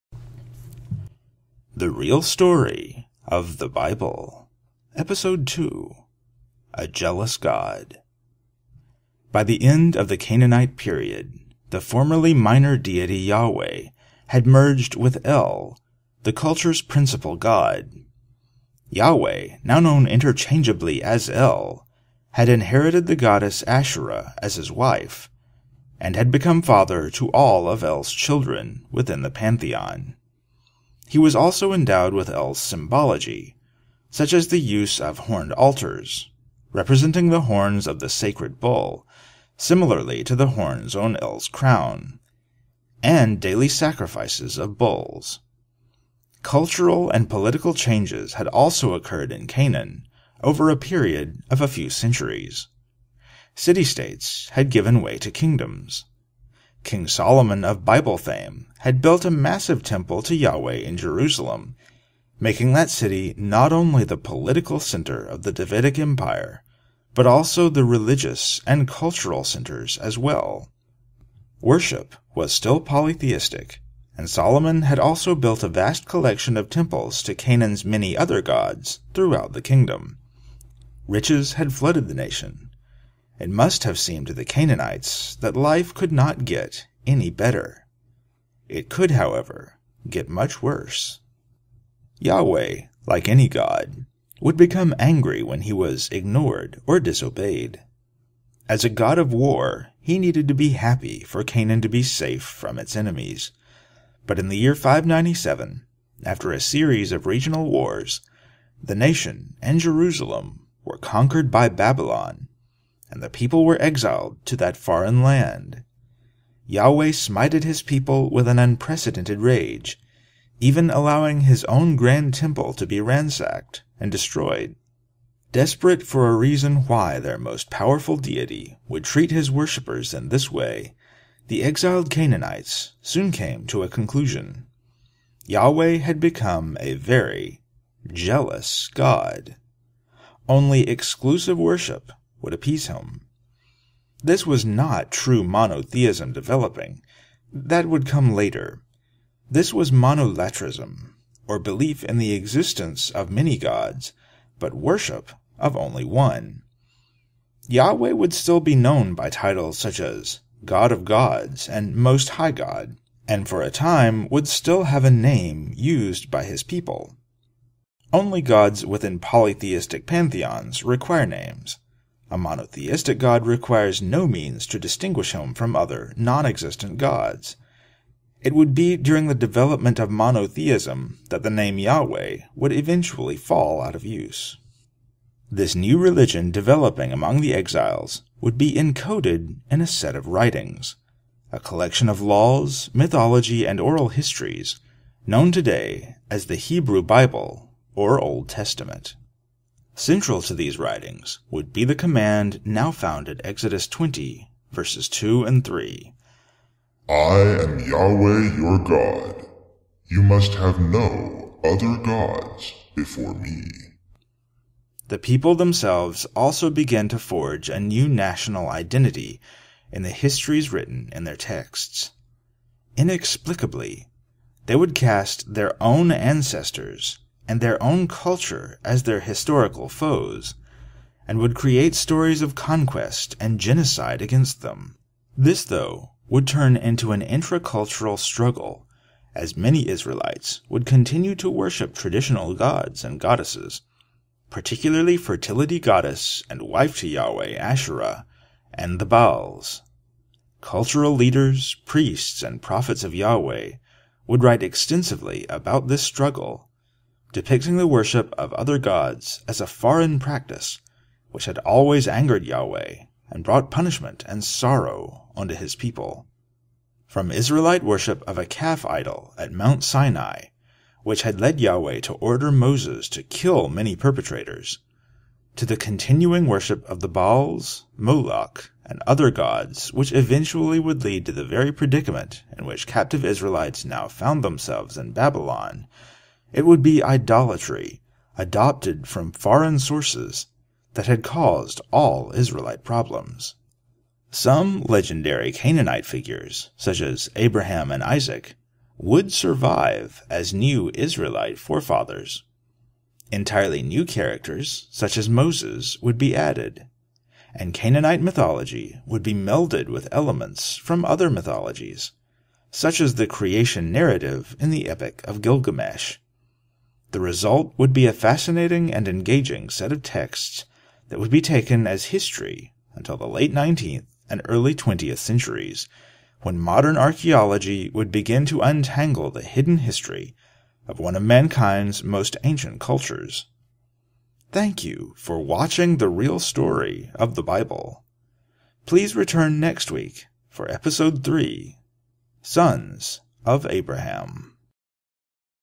The Real Story of The Bible: A Jealous God
Video 2 in the series the Real Story of The Bible, in which I discuss the end of the Canaanite and beginning of the Israelite cultures, and how monotheism began.